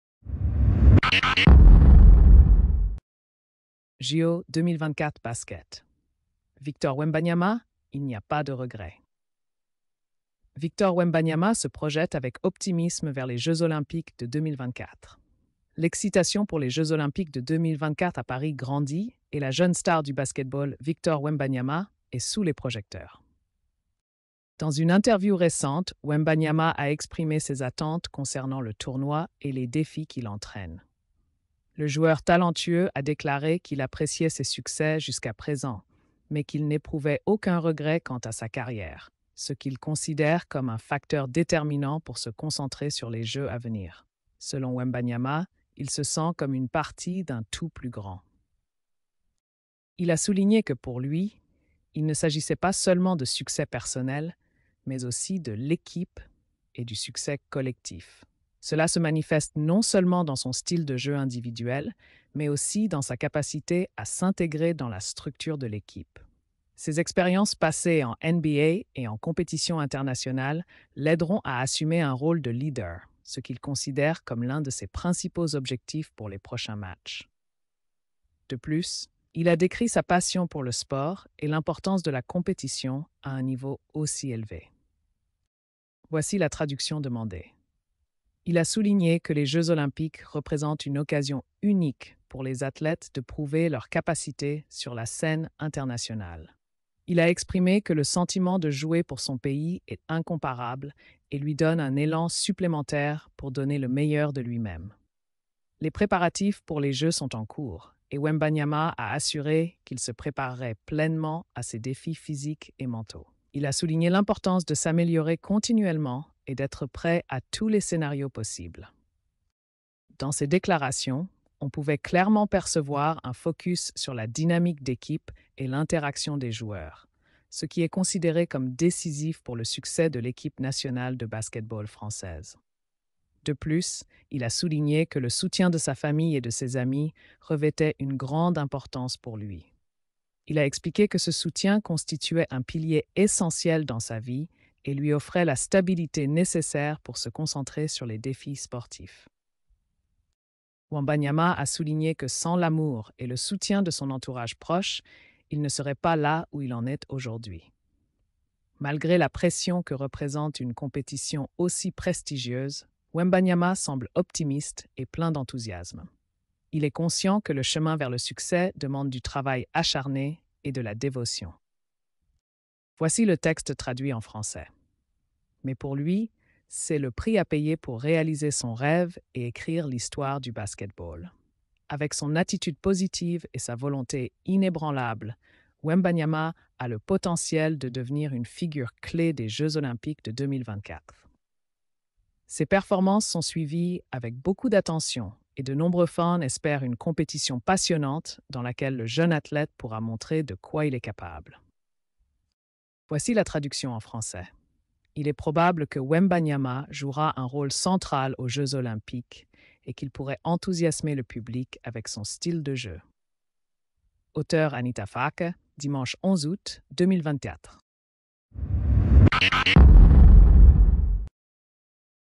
Victor Wembanyama optimiste pour les JO 2024 sans regrets. (Podcast)
Victor Wembanyama est optimiste pour les JO 2024 à Paris. AI-generated content